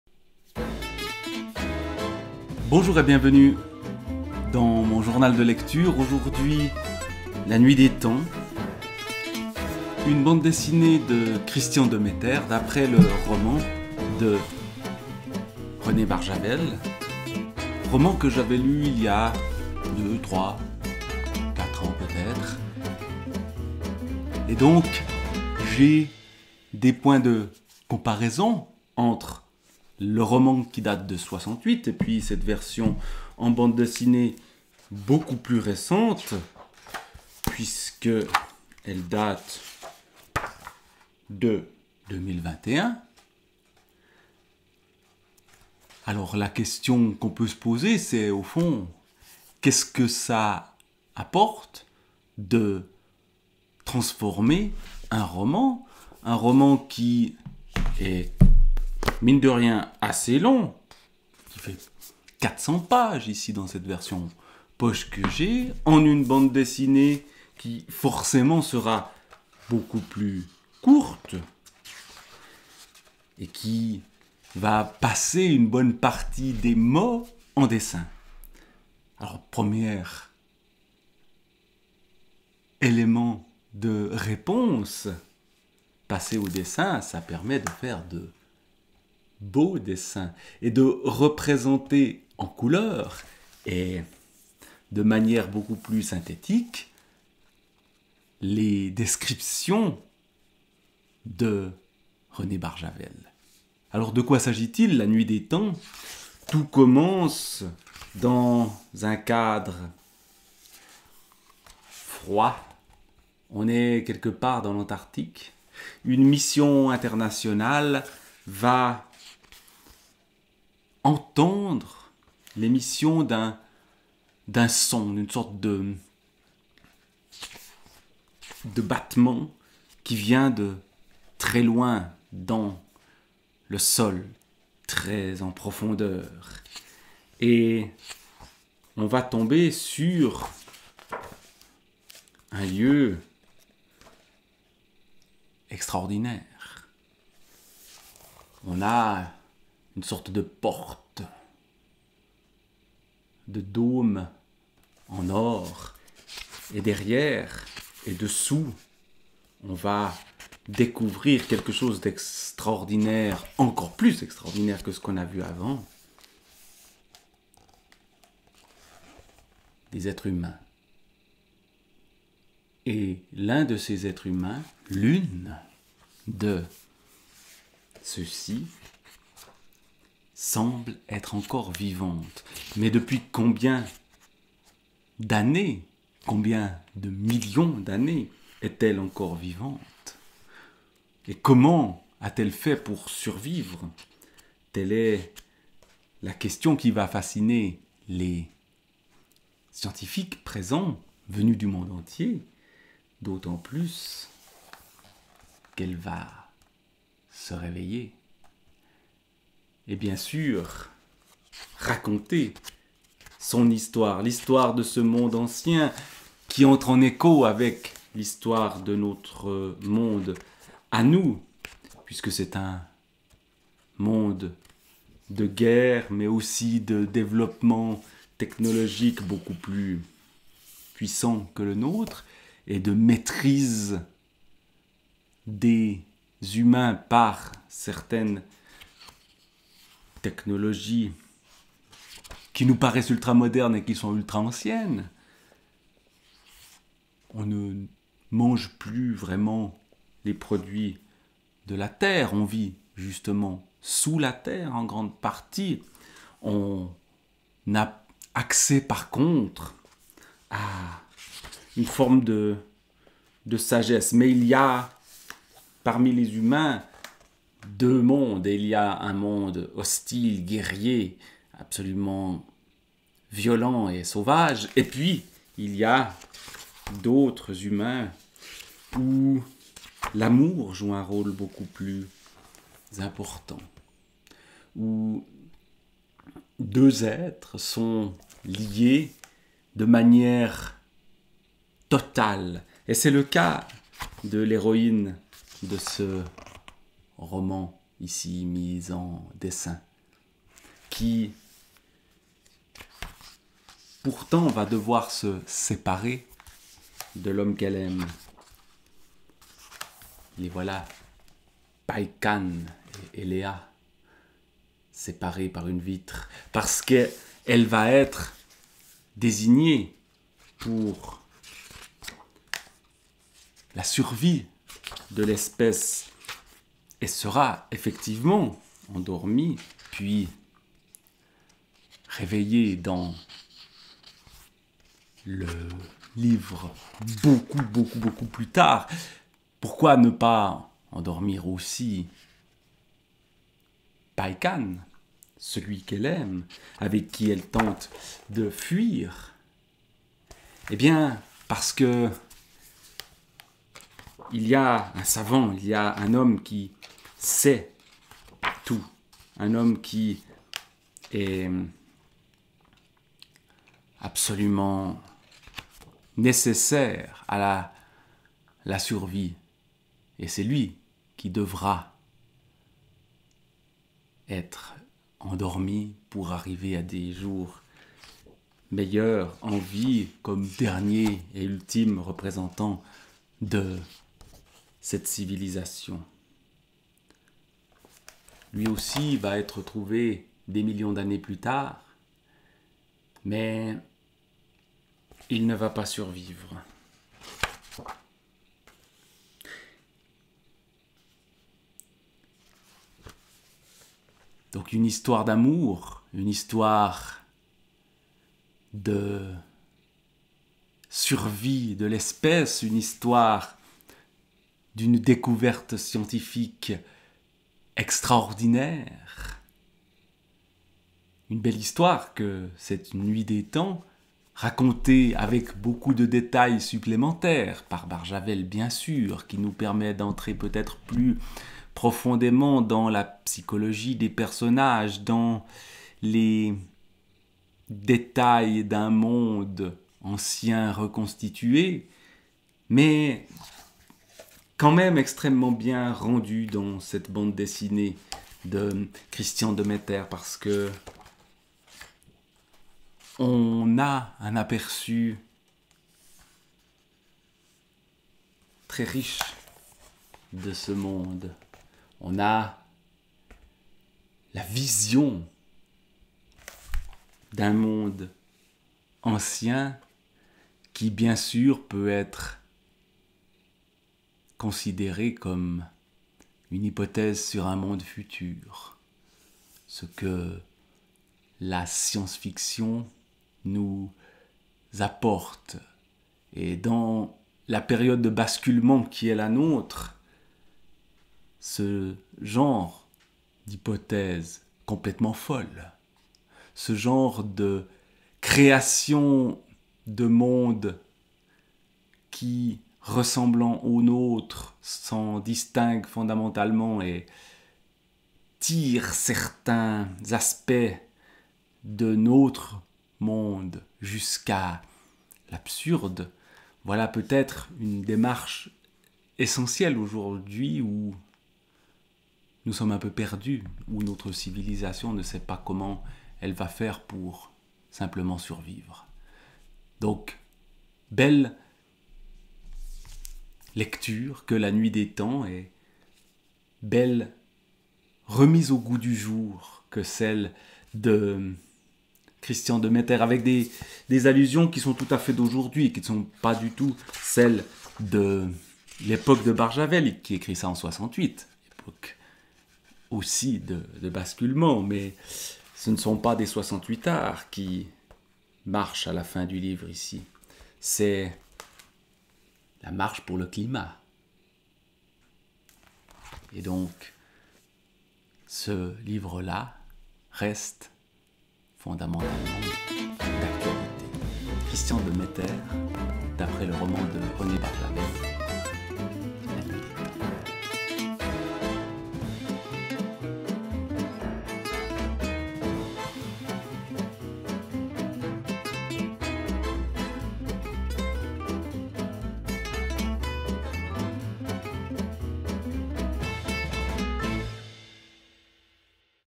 Christian de Metter - La Nuit des Temps
Dans le cent-septante-troisième épisode de mon journal de lecture, je feuillette La Nuit des Temps, une bande dessinée de Christian de Metter publiée en 2021 aux édtions Philéas d'après le roman éponyme de René Barjavel, écrit en 1968.